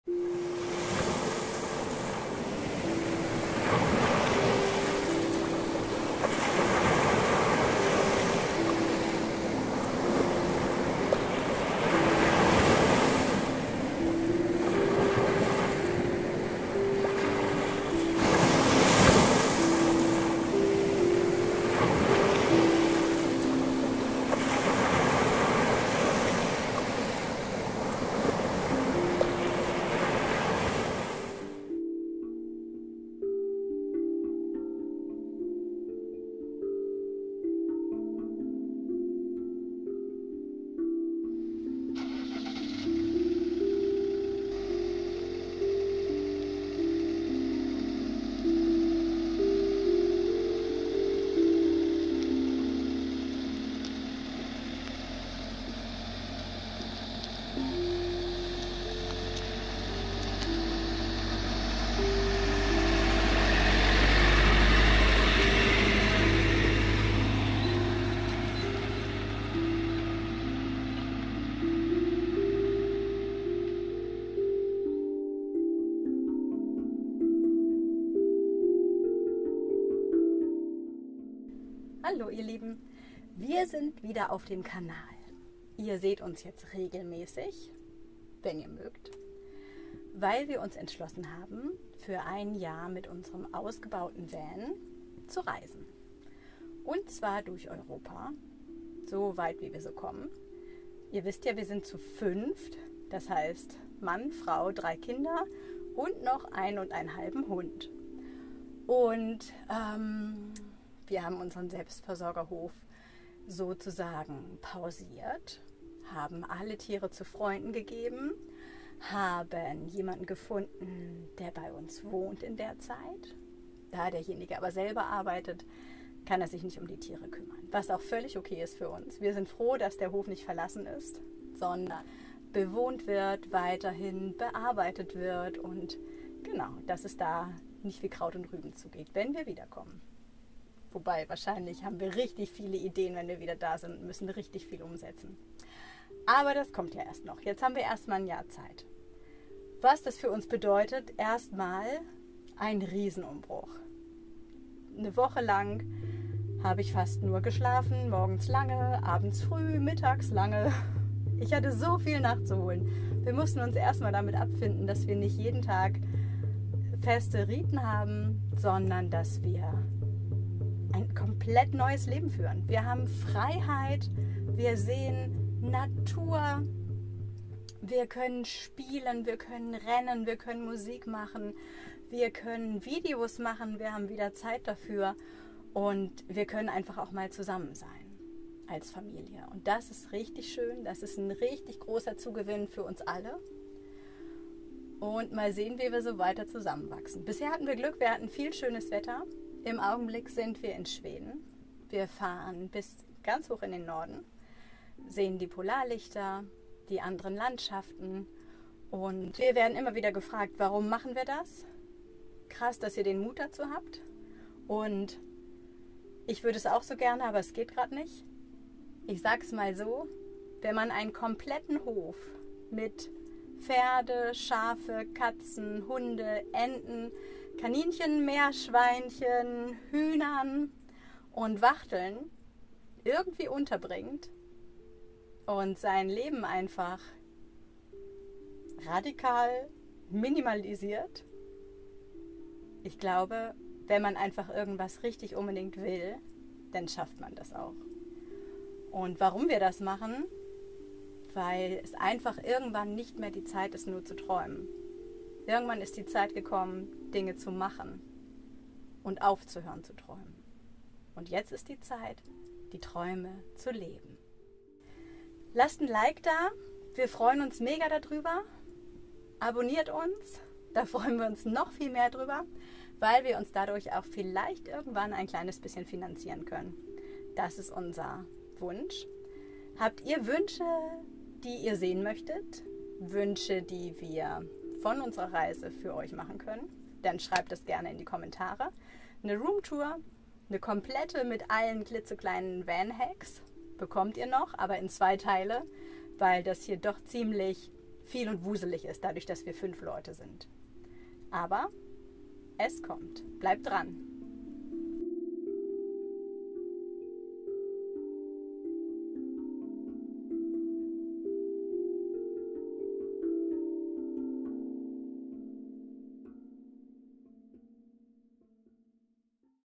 1Jahr Vanlife als Familie, zu fünft im Van durch Europa
Wir fahren los, in unser neues Leben, in ein ganz großes Abenteuer. Seid mit uns dabei 🍀